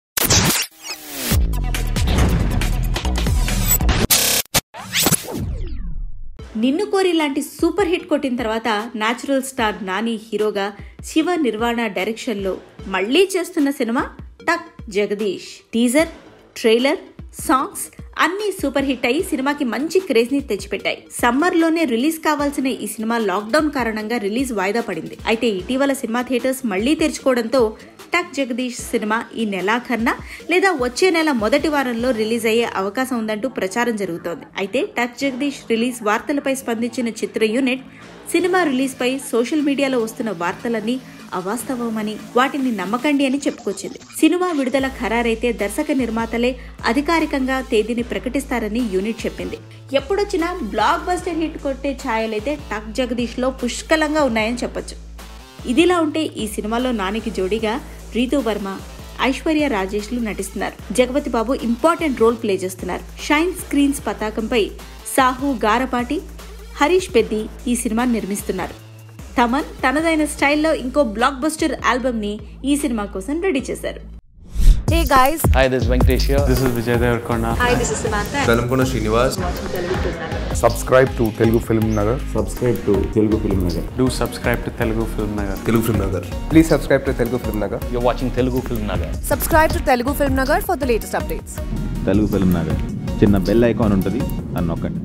Tuck Jagadish Release Date Update | Nani | Ritu Varma | Aishwarya Rajesh | Thaman | Shiva Nirvana
Tuck Jagadish Movie Release Date Update, 2021 Latest Telugu Movie Updates on Telugu FilmNagar. TuckJagadish 2021 latest Telugu movie ft. Nani, Ritu Varma, and Aishwarya Rajesh. Directed by Shiva Nirvana and produced by Sahu Garapati and Harish Peddi. Music Composed by Thaman S.